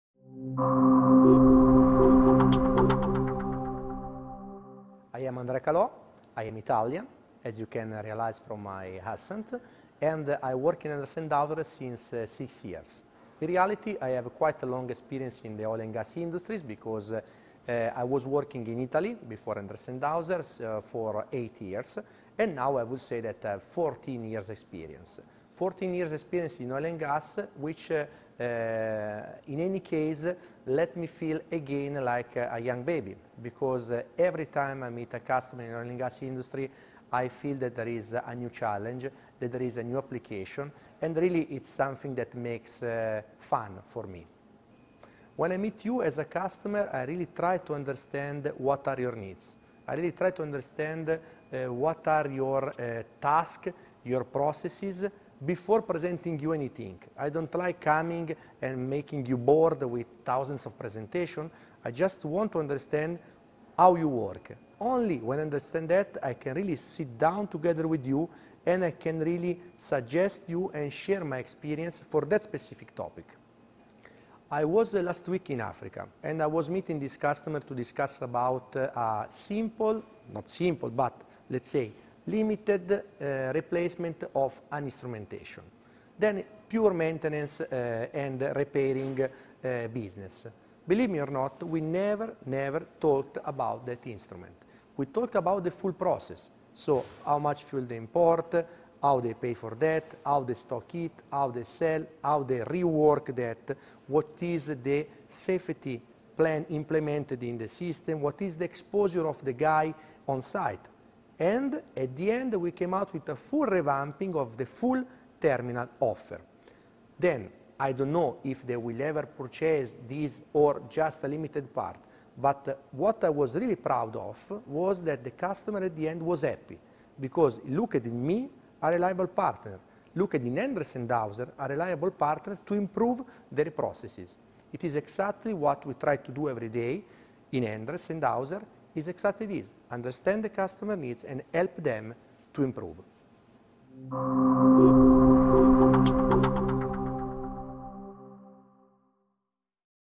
Meet Andrea Caló, Oil & Gas industry expert at Endress+Hauser
…what I was really proud of was that at the end the customer was happy because he looked at me as a reliable partner, he looked at Endress+Hauser as a reliable partner to improve their processes.